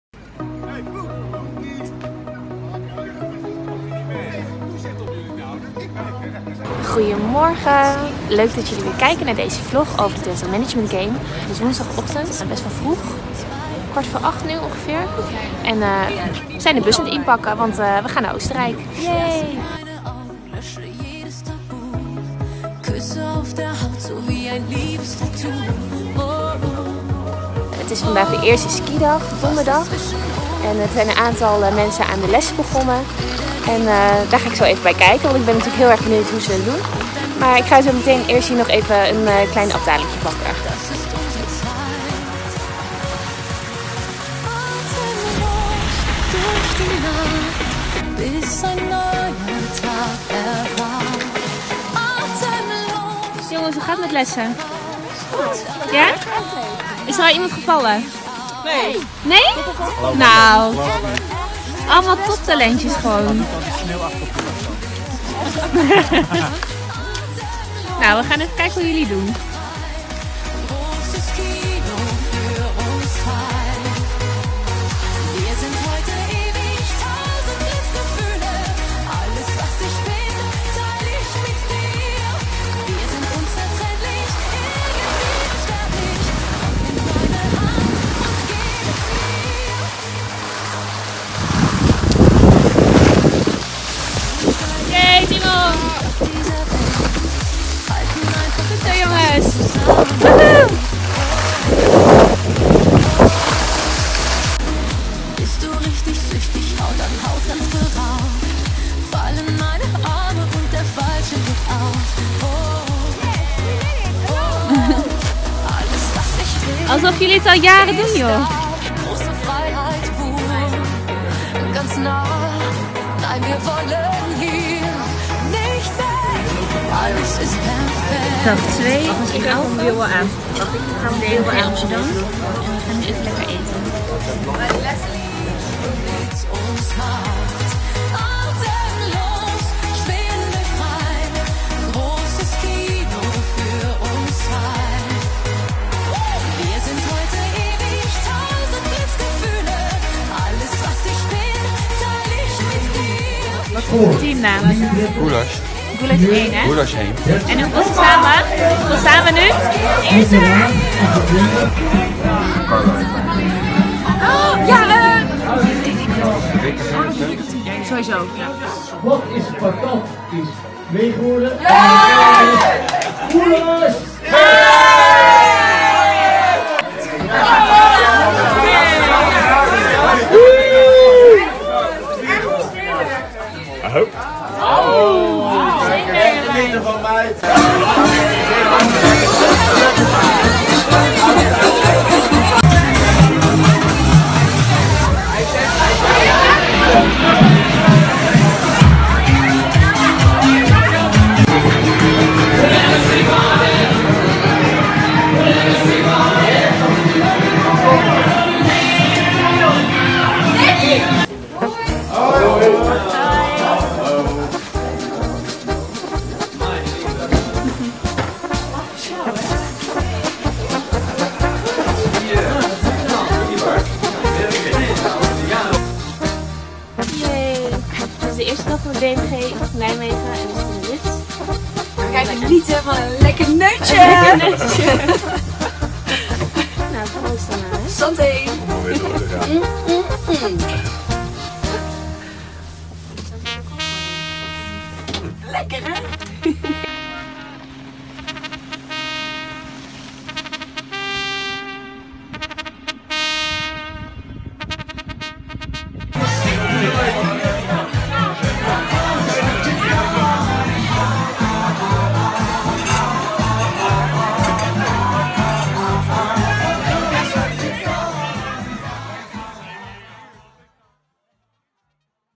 DMG-studenten op de latten | Dental Management Game | Vlog 6
Als afsluiting van de DMG-bijeenkomsten gingen de studenten tandheelkunde een paar dagen skiën in Oostenrijk.